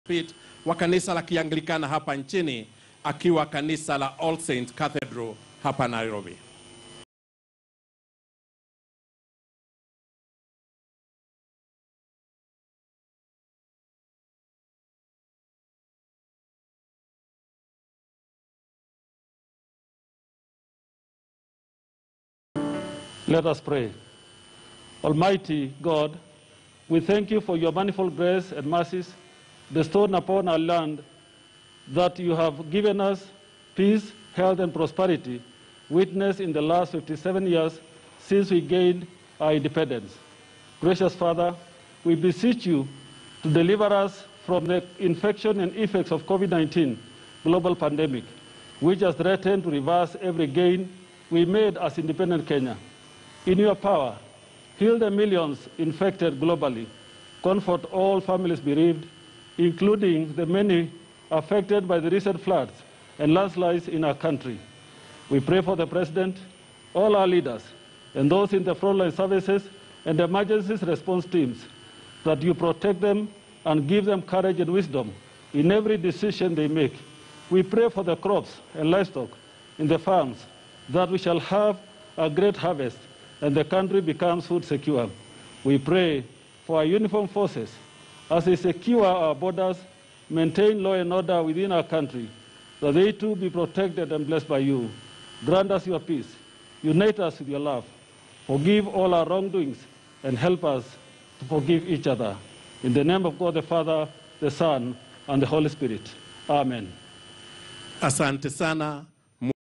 Jackson ole sapit prayers during 57th Madaraka day celebration
KTN NEWS- 57TH Madaraka Day Celebrations